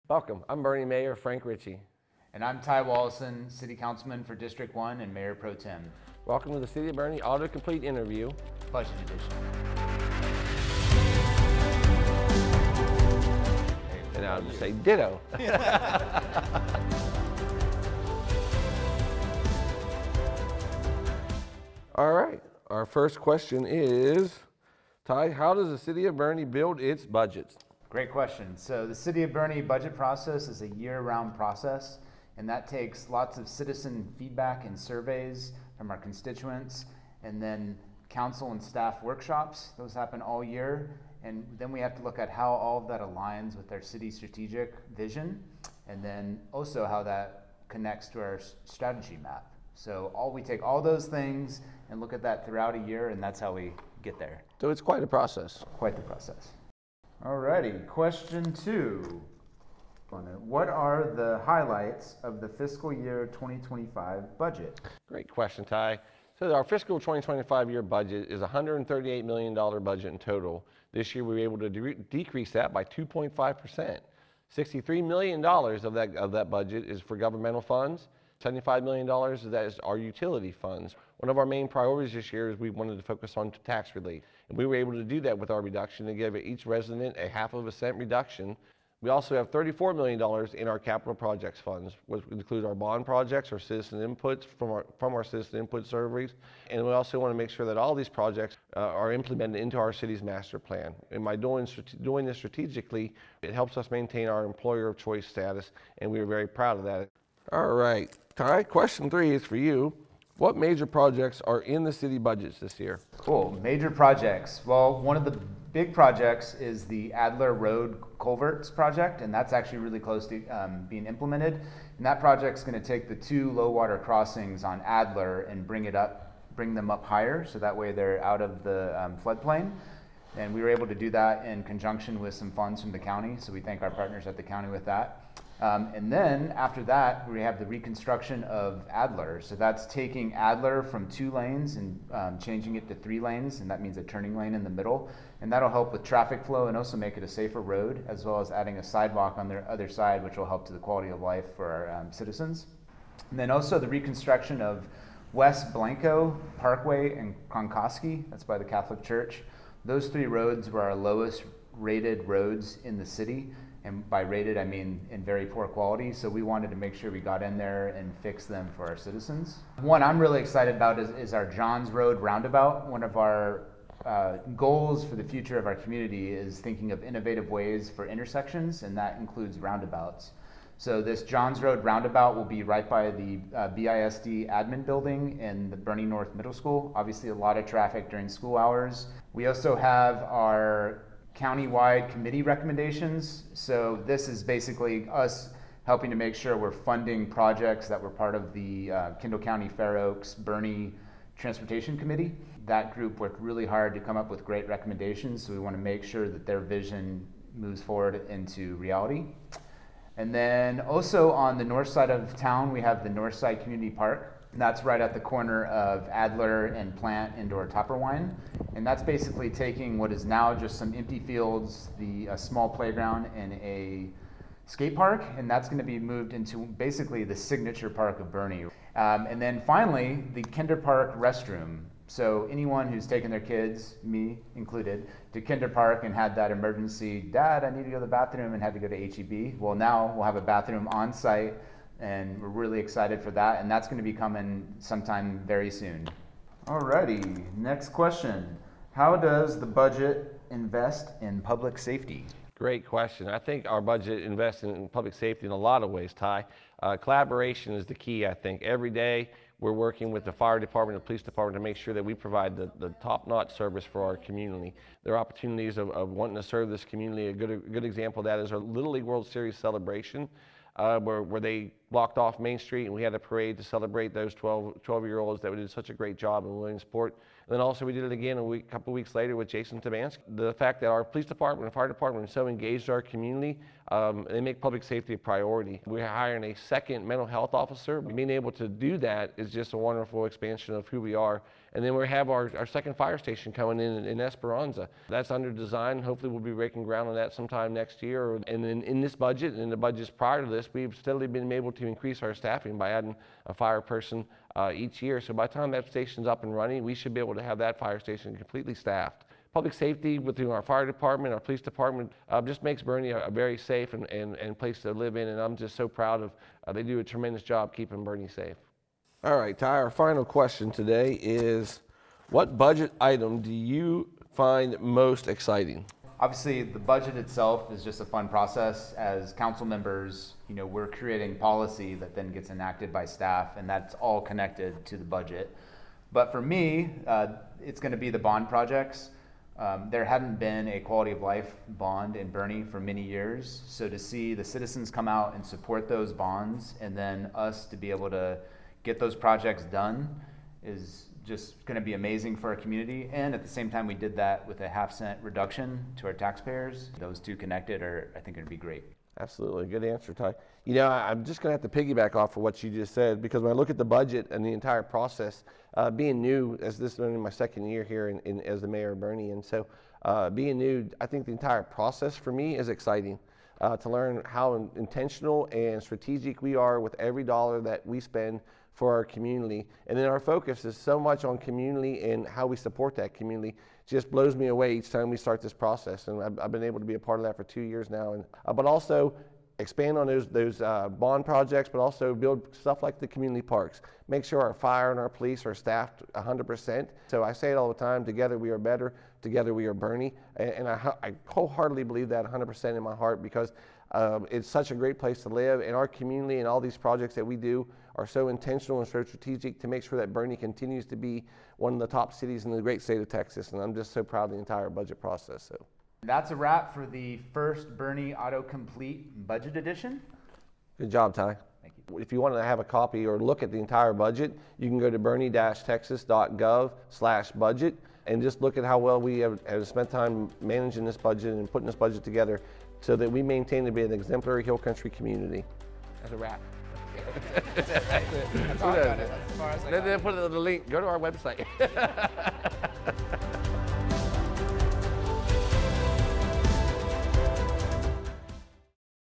Autocomplete Interview: Budget Edition
You could say Mayor Frank Ritchie and Mayor Pro-tem Ty Wolosin are just ‘wired’ a little differently. They join us and discuss the newly adopted Fiscal Year 2025 budget! This year’s budget delivers key priorities such as:

Tax relief
Strategic infrastructure investment
Employee investment
Security and Safety

With a half-cent tax rate reduction, the City continues to invest in streets and parks upgrades, allowing Boerne to thrive. Watch the video for more on how the budget supports implementation of the 2022 bond projects, strategic capital improvements, public safety, streets and other quality of life topics.